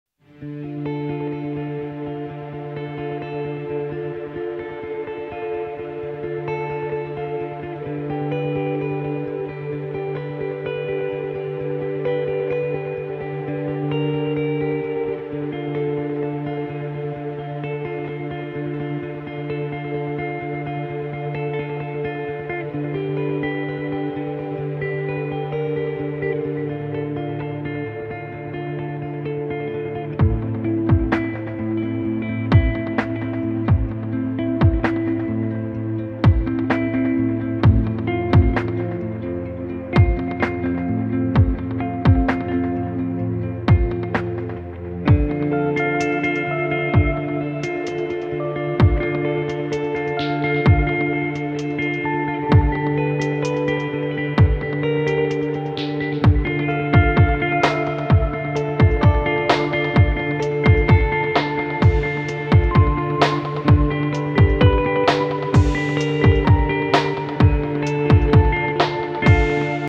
Kuthodaw Pagoda, Myanmar
The photographs were taken late in the day, just before sunset.